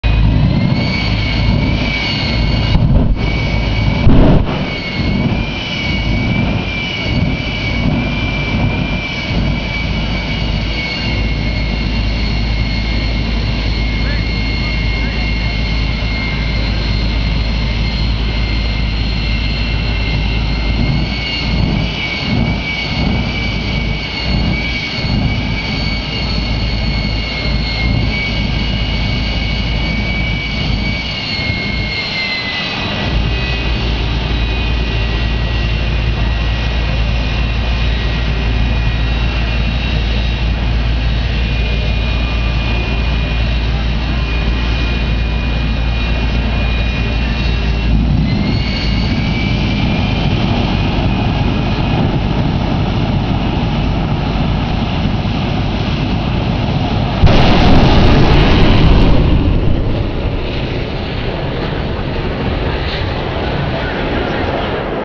Jet Cars Under The Stars 4
Part two of the Jet Truck's run. Standing on the top of the announcer's tower with stuff in the way was difficult to get in one sweep.